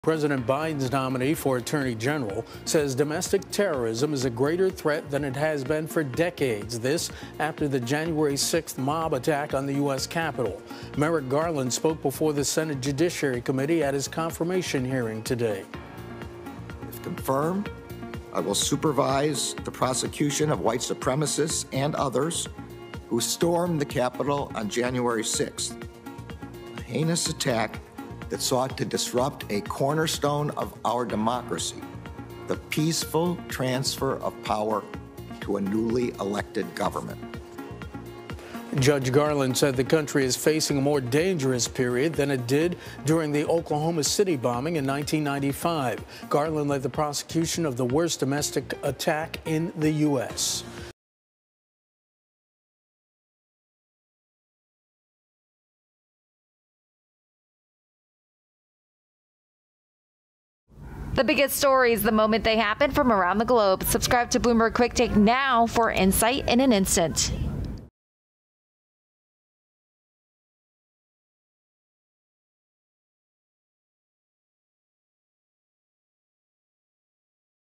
Merrick Garland Says Capitol Riot Shows Domestic Terrorism Is Rising
Merrick Garland, President Joe Biden’s nominee for attorney general, said domestic terrorism is a greater threat than it has been for decades after the Jan. 6 mob attack on the U.S. Capitol.

“We are facing a more dangerous period than we faced in Oklahoma City,” said Garland, who led the prosecution of the worst domestic attack in the U.S., the truck bombing of the federal building there in 1995.

Garland, who’s now a judge on the U.S. Court of Appeals for the District of Columbia, appears headed for a bipartisan vote of approval in the Senate. Senator Chuck Grassley of Iowa, the committee’s top Republican, called Garland “a good pick to lead the Department of Justice,” and Republican Senator Lindsey Graham said he’s “very inclined” to support him.

As the Justice Department pursues criminal cases stemming from the Capitol riot, Garland pledged to lead the effort. “If confirmed, I will supervise the prosecution of white supremacists and others who stormed the Capitol on Jan. 6 -- a heinous attack that sought to disrupt a cornerstone of our democracy: the peaceful transfer of power to a newly elected government,” he said.

Garland also signaled he’ll make decisions independently from Biden. “The president nominates the attorney general to be the lawyer -- not for any individual, but for the people of the United States,” he said.